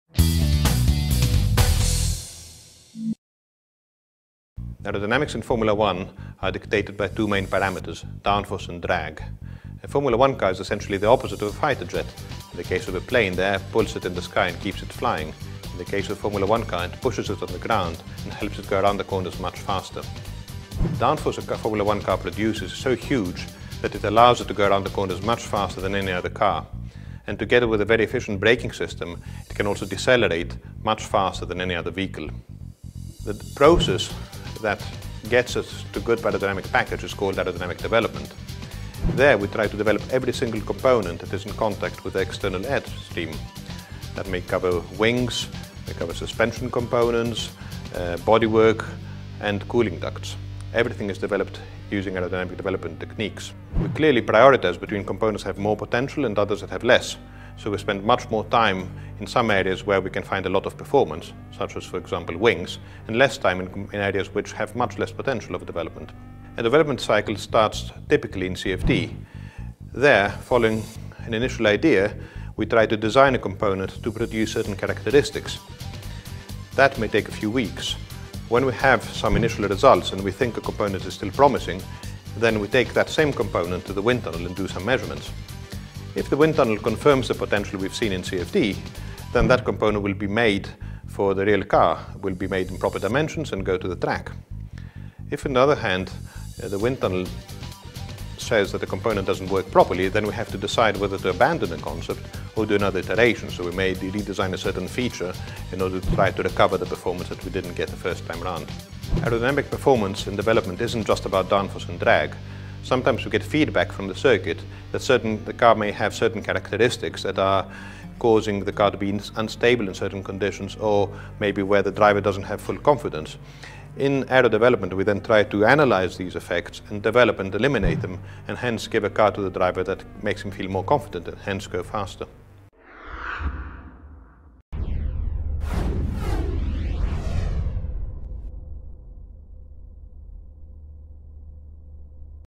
Ferrari F1 Tech Review - Aerodynamic development
Scuderia Ferrari F1 Tech Review -- Aerodynamic development

Demonstrator Mr Tombazis.

By Ferrari S.p.A.